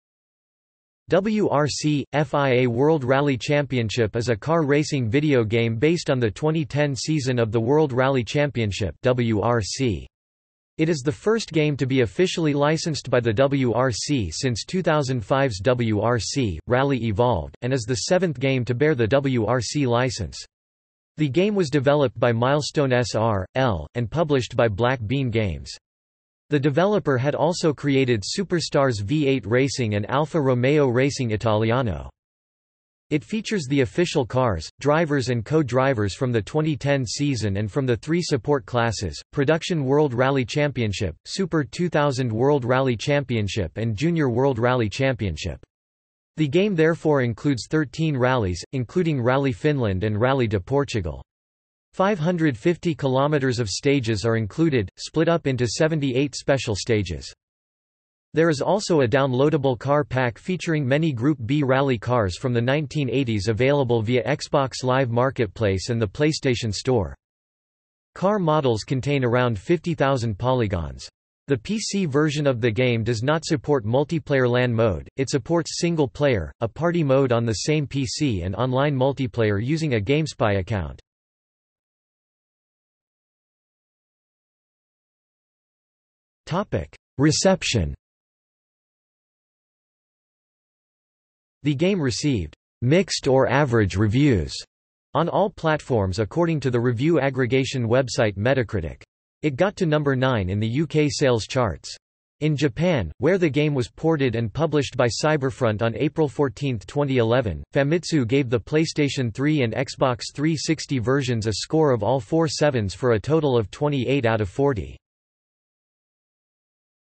WRC: FIA World Rally Championship (2010 video game) | Wikipedia audio article
00:01:22 Reception

- Socrates

SUMMARY
WRC - FIA World Rally Championship is a car racing video game based on the 2010 season of the World Rally Championship (WRC). It is the first game to be officially licensed by the WRC since 2005's WRC: Rally Evolved, and is the seventh game to bear the WRC licence. The game was developed by Milestone S.r.l. and published by Black Bean Games. The developer had also created Superstars V8 Racing and Alfa Romeo Racing Italiano.
It features the official cars, drivers and co-drivers from the 2010 season and from the three support classes: Production World Rally Championship, Super 2000 World Rally Championship and Junior World Rally Championship. The game therefore includes 13 rallies, including Rally Finland and Rally de Portugal. 550 km of stages are included, split up into 78 special stages.
There is also a downloadable car pack featuring many Group B rally cars from the 1980s available via Xbox Live Marketplace and the PlayStation Store.
Car models contain around 50,000 polygons. The PC version of the game does not support Multiplayer LAN mode, it supports single player, a party mode on the same PC and Online Multiplayer using a GameSpy account.